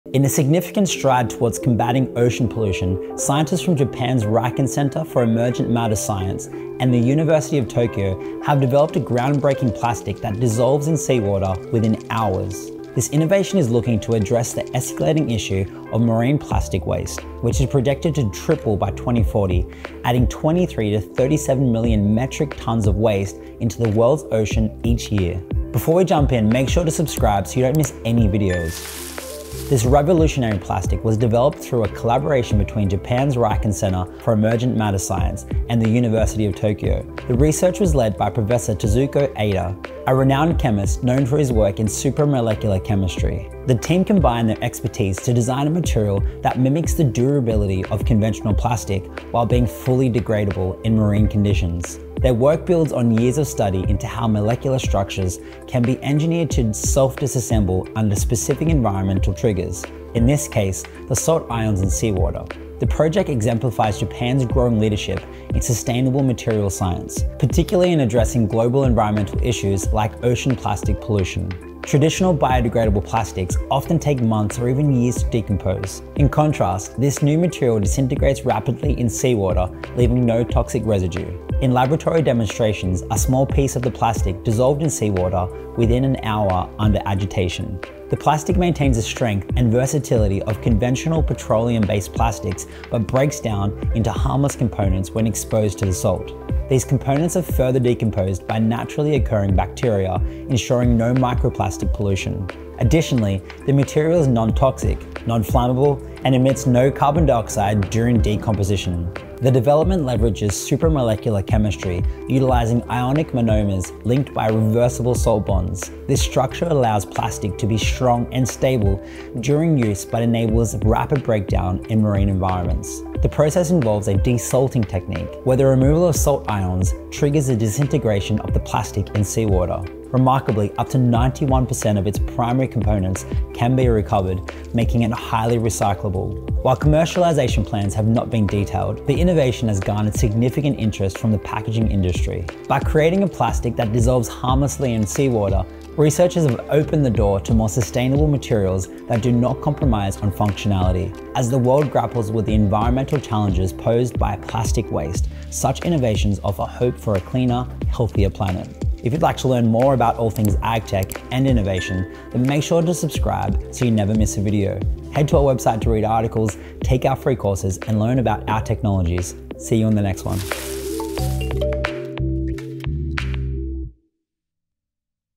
The Plastic That Dissolves in Seawater!
In a significant stride toward combating ocean pollution, scientists from Japan's RIKEN Center for Emergent Matter Science and the University of Tokyo have developed a groundbreaking plastic that dissolves in seawater within hours.